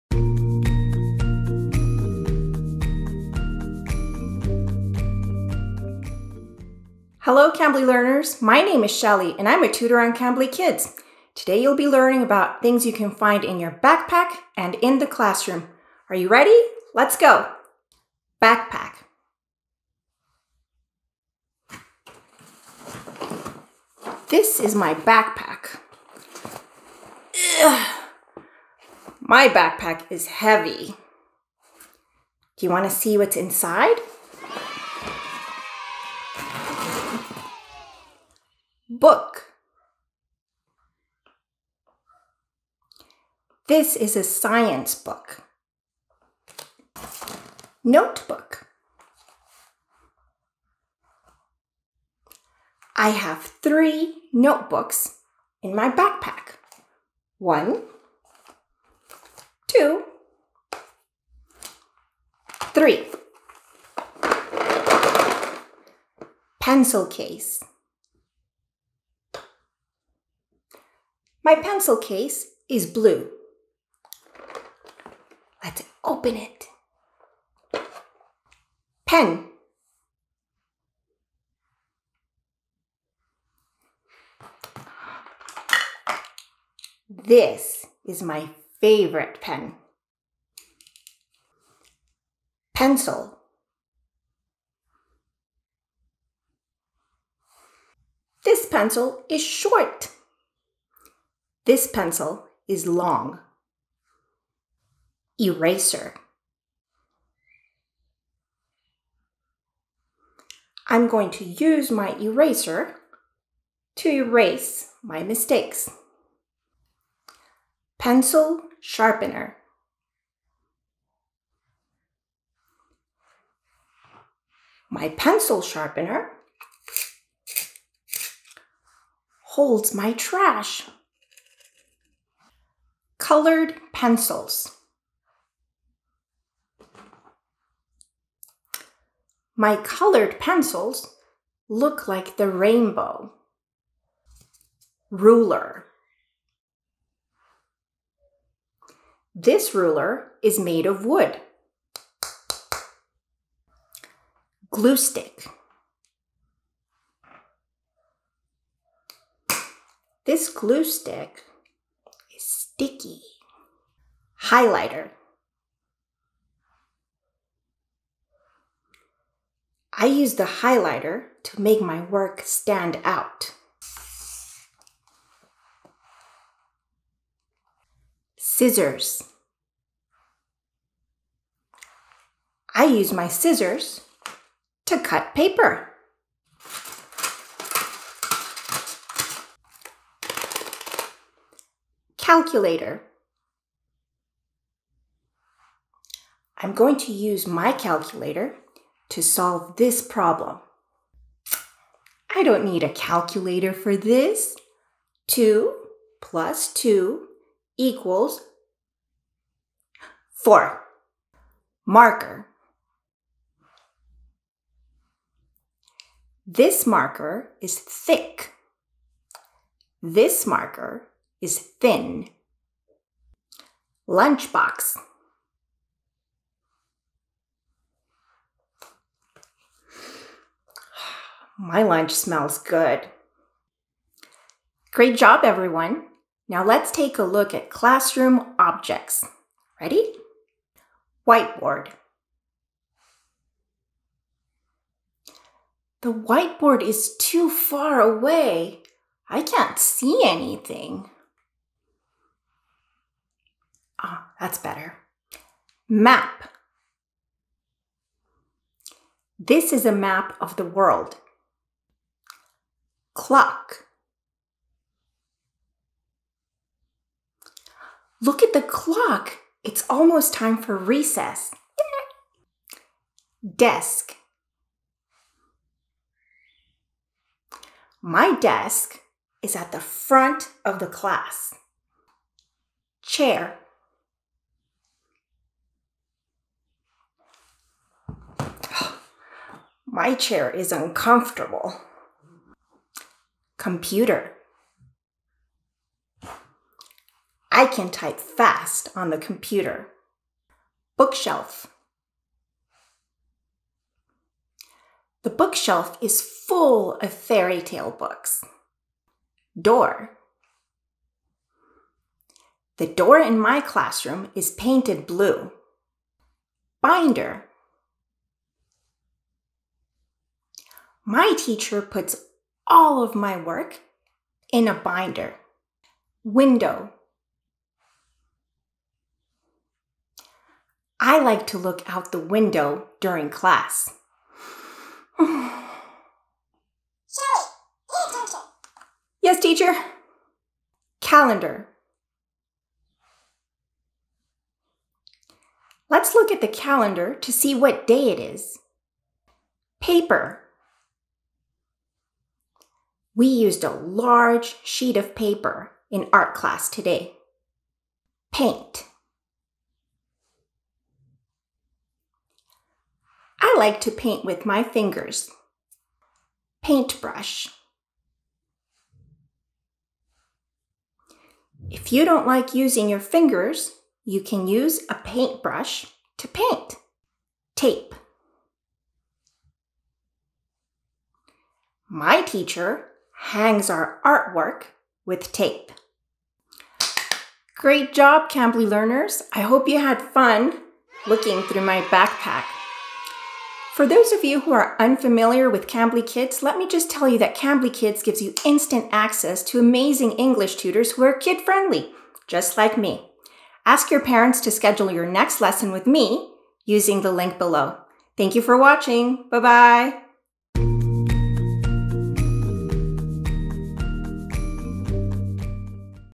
Things in Your Backpack & the Classroom
In this lesson, learn about the things you can find in your backpack and your classroom.